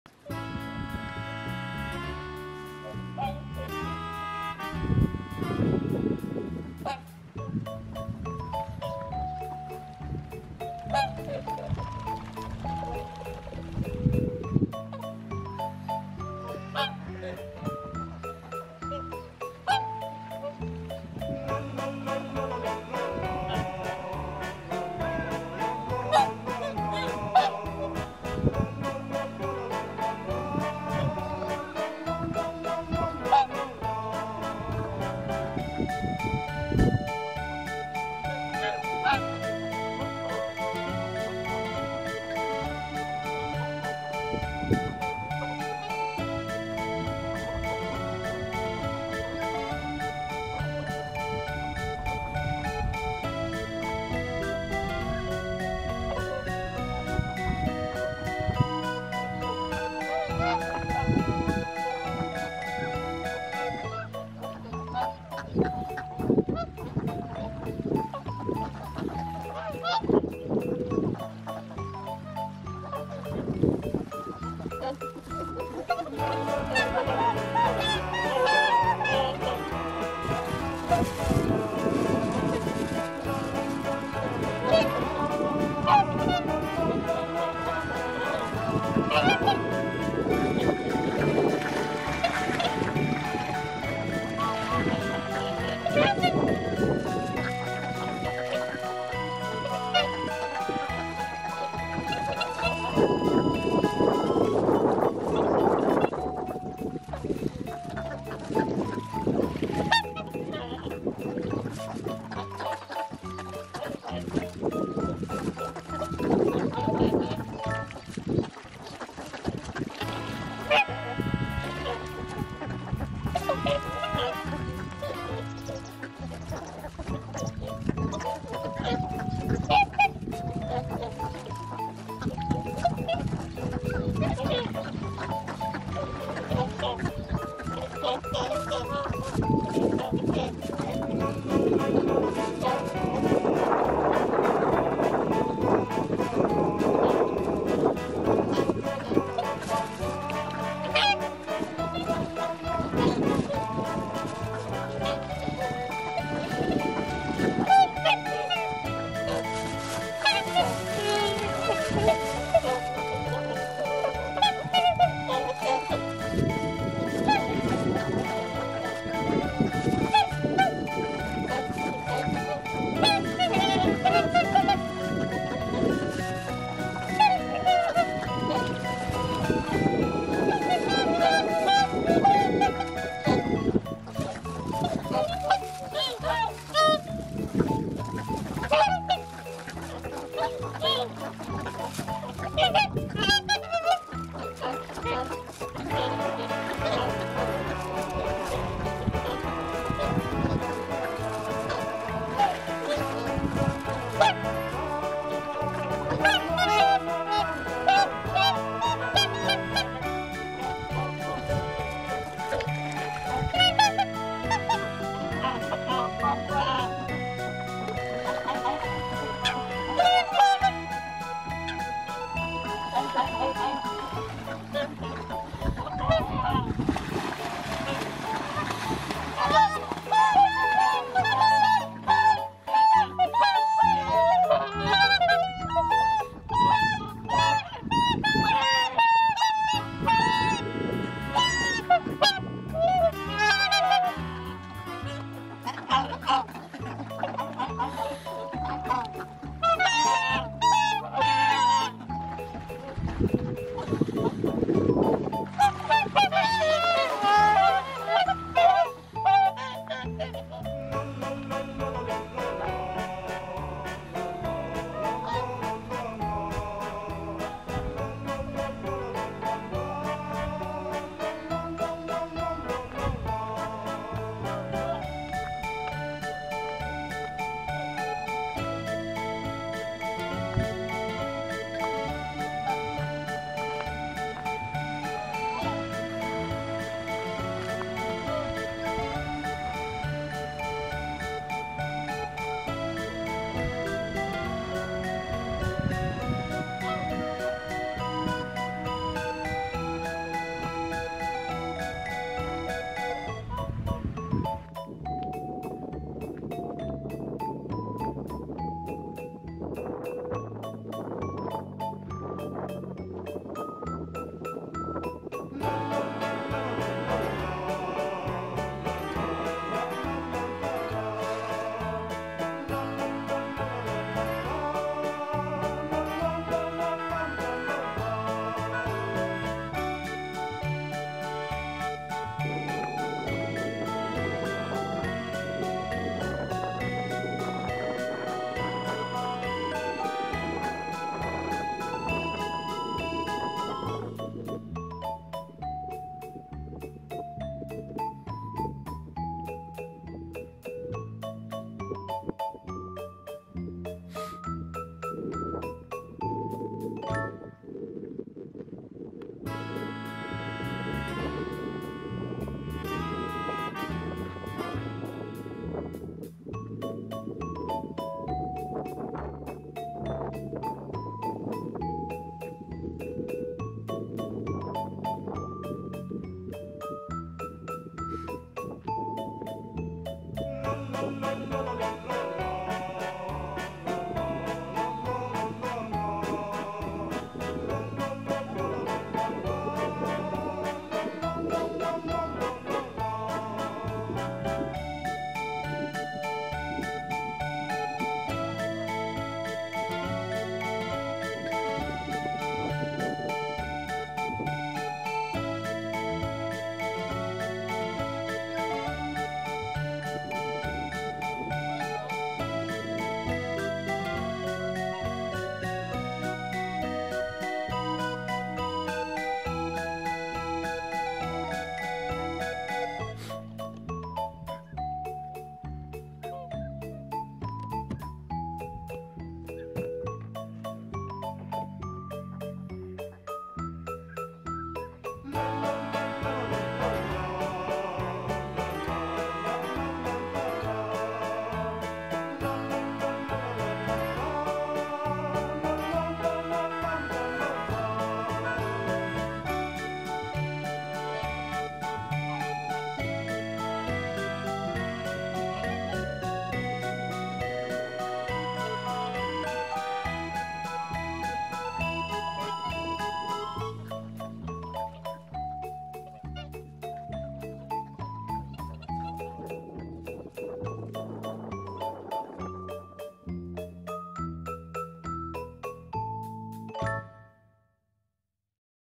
【福島県楢葉町】白鳥の飛来地へ行ってきた【上繁岡大堤】
アンモナイトセンターでの体験発掘の帰りに、

福島県楢葉町にある白鳥の飛来地へ行ってきました。

最盛期ほど白鳥さんは集まらないようですが
水辺へ行くと鳥さんたちが迎えてくれます。

12月に入ったあたりから白鳥さんの姿が見られるようになりましたが、
動画を撮った現在で30羽ほどでしょうか・・・
今年春の北帰行前には100羽ほどいたと思いますが、
果たして今年はどれくらい白鳥さんは飛来するでしょうか・・・？

BGM・SE
YouTubeオーディオライブラリー